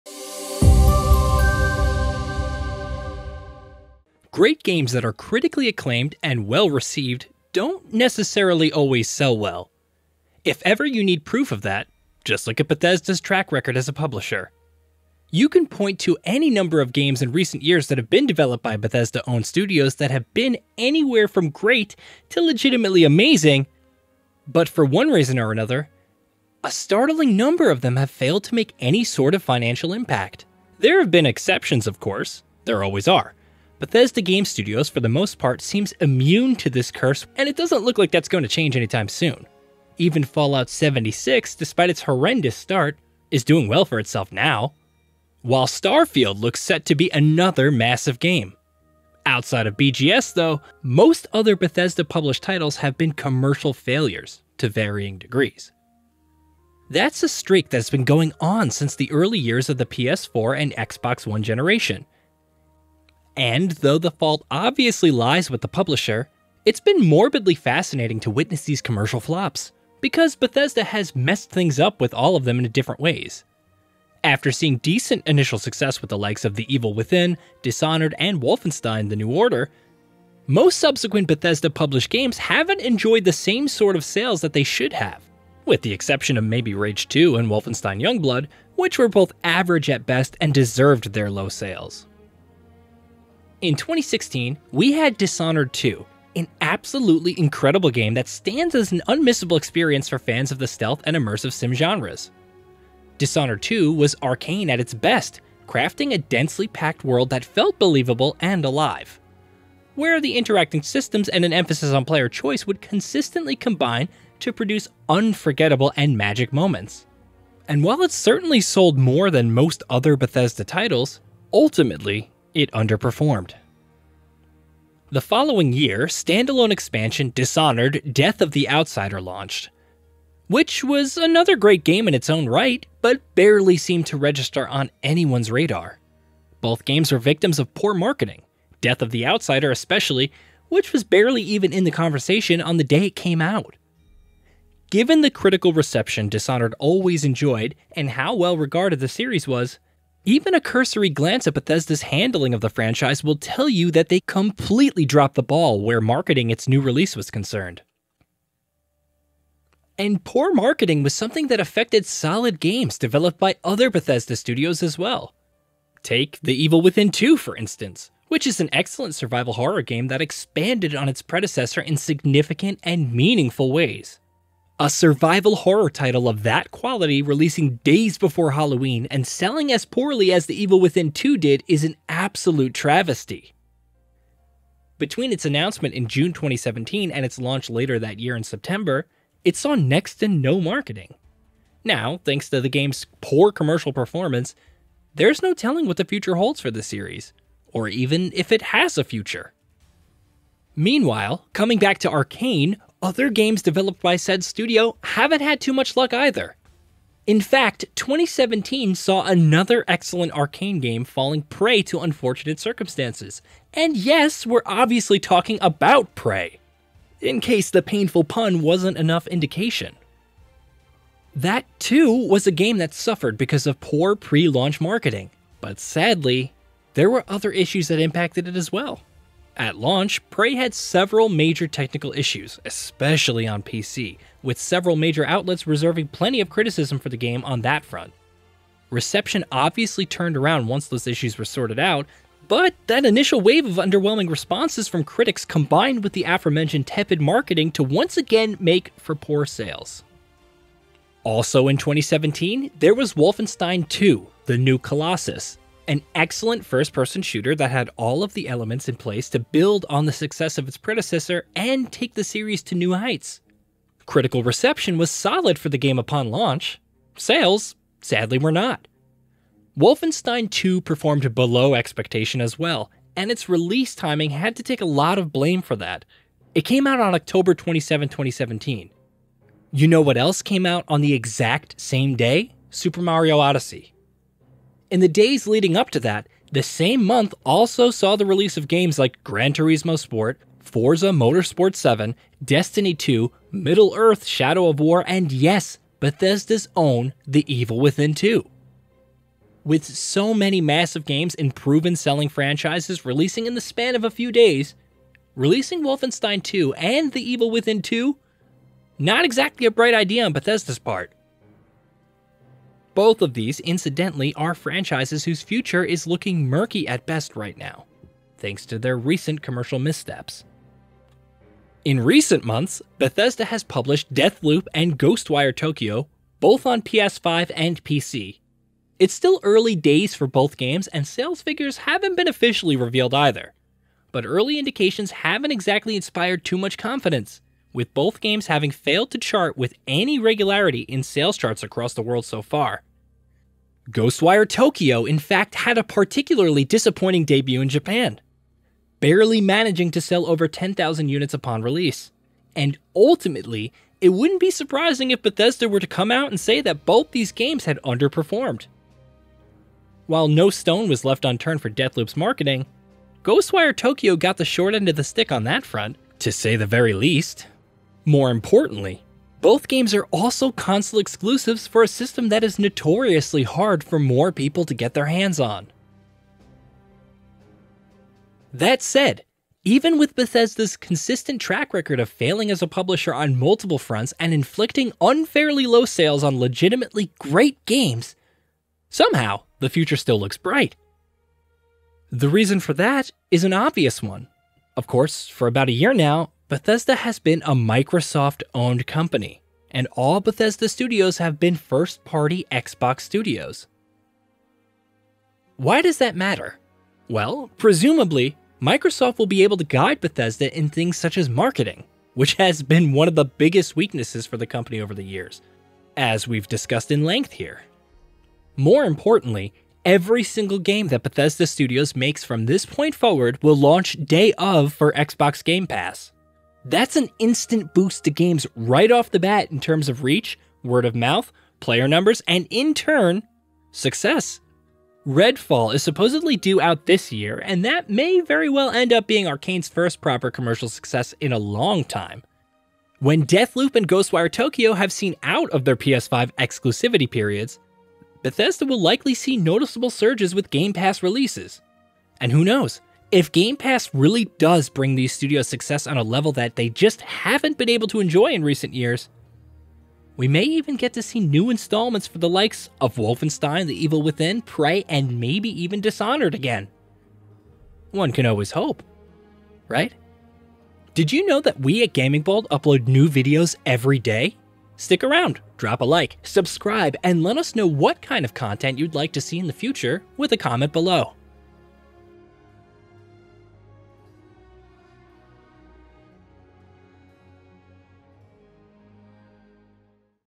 What’s Up With Bethesda Published Single Player Games Flopping Commercially?
Bethesda-published games often see a startling disparity in critical reception and commercial performances- why exactly is that?